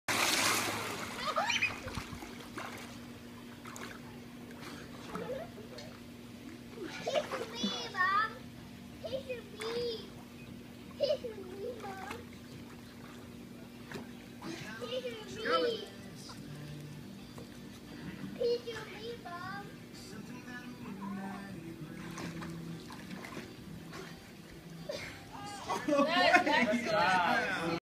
Lucy swimming
4 years old swimming without floaties!!